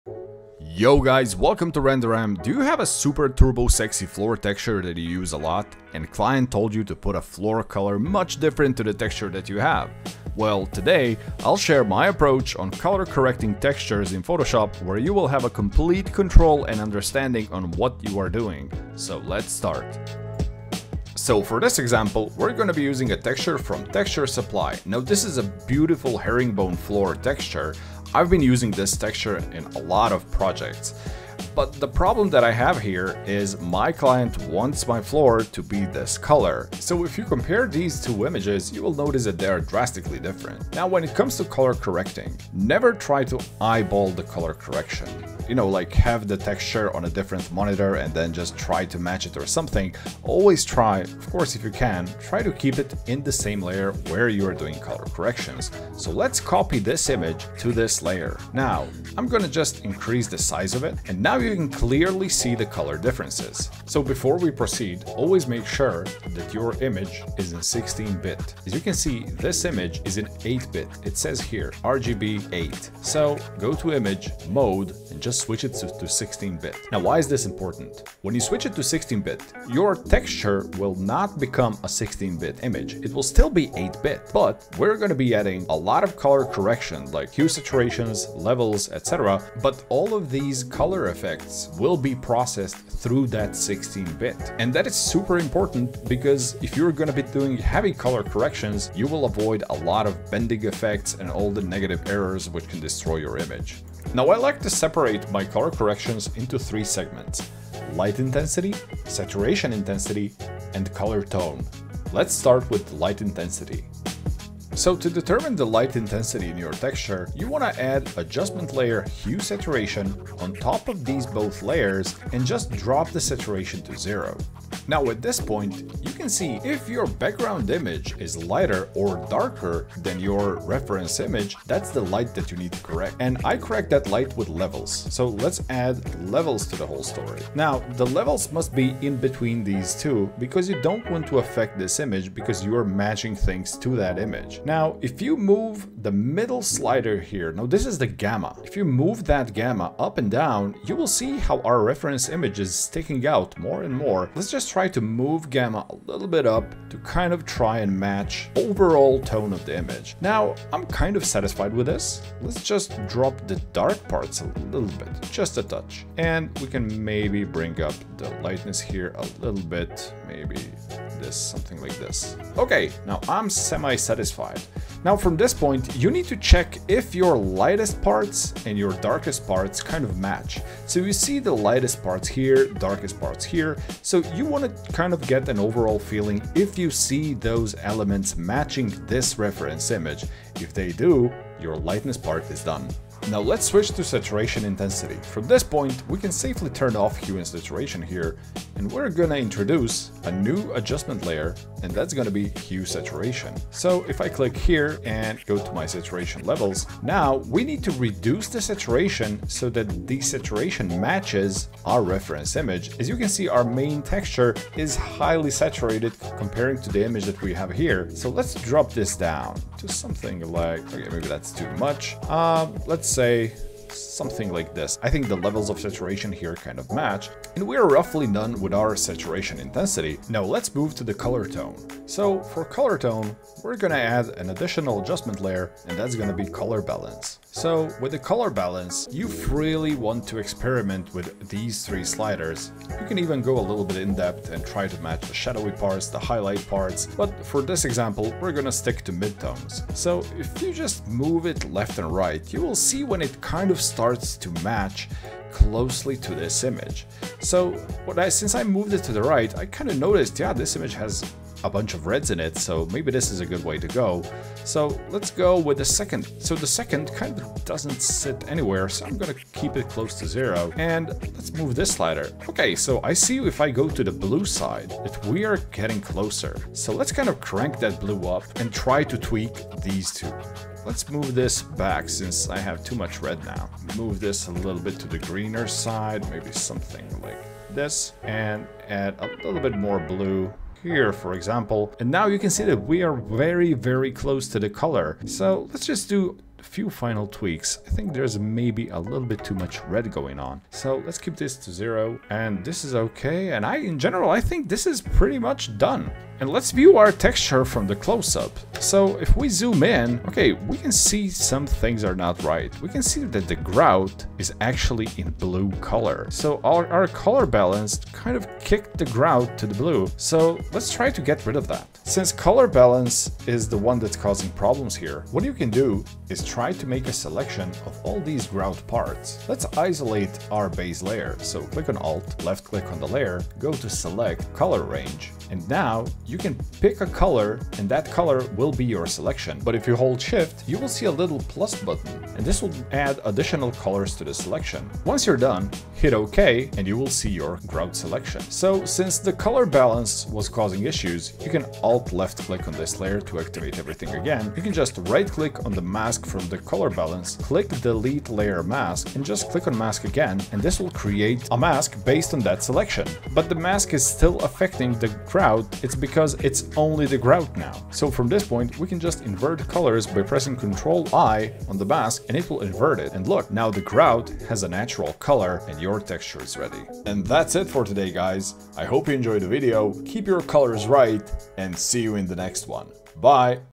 Completely Control Texture Color Matching | All 2D Software
Yo guys, on @RenderRam  I'm gonna cover a quick color correction method with textures, where you'll understand what is important when matching colors and how to approach it!

00:00 Intro
01:52 Light Intensity
03:14 Saturation Intensity
03:51 Color Tone
05:09 Grout color fix